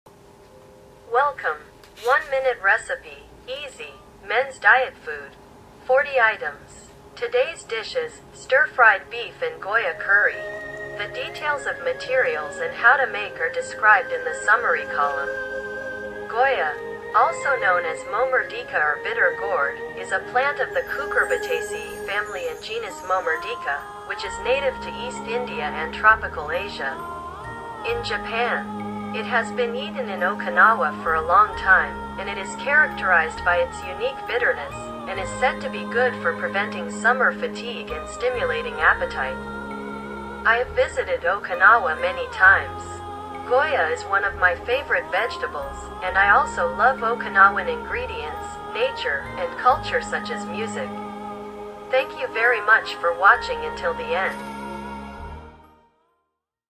Welcome! 1 minute recipe, easy, men's diet food! 40 items.
Today's dish is (stir-fried beef and goya curry).

Goya, also known as Momordica or bitter gourd, is a plant of the Cucurbitaceae family and genus Momordica, which is native to East India and tropical Asia.

As you can see from the place of origin, it can be found in warm regions, and in Japan it has been eaten in Okinawa for a long time.

Characterized by its unique bitterness, it is a vegetable that is said to be good for preventing summer fatigue and stimulating appetite.

I love Okinawa and have visited many times.

The first time I met bitter gourd was at my senior's house in Okinawa.

In fact, there was a time when we decided to move to Okinawa several years ago.

However, her mother-in-law's condition suddenly changed and she gave up.

I hate the cold, so in the future, I hope to be able to travel to warmer regions, even if only for the winter.

That's why I love Okinawa's ingredients, nature, and culture such as music.

(Material)

Beef 100g
1 bitter gourd
1 spring onion
1 piece of ginger
For seasoning the beef (salt, pepper, a little bit of each)
1 tablespoon of sesame oil
salt, pepper, a little each
1 tablespoon of curry powder
1 teaspoon of soy sauce

①Cut the beef into bite-sized pieces and marinate.
② Cut the goya in half lengthwise, scrape off the seeds and cotton with a spoon, cut diagonally, sprinkle with salt (not listed), leave for about 15 minutes, rinse with water and drain.
③ Cut the spring onion and ginger into strips.
④Heat a frying pan, add sesame oil, stir-fry ginger and green onion, then add beef and stir-fry. When the color changes, add bitter gourd and stir-fry. When bitter gourd is cooked, season with salt, pepper, curry powder, and soy sauce, and it's done.

Thank you very much for watching until the end.